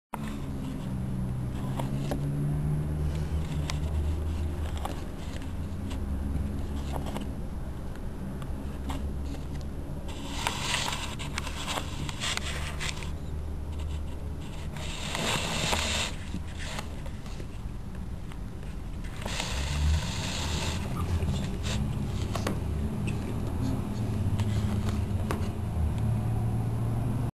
my turtle it´s so funny!! 100% tortuga argentina!
My turtle trying to eat a tomato behind some plastic protection. Don´t miss it!!!

LOL! OMG! tomate tomato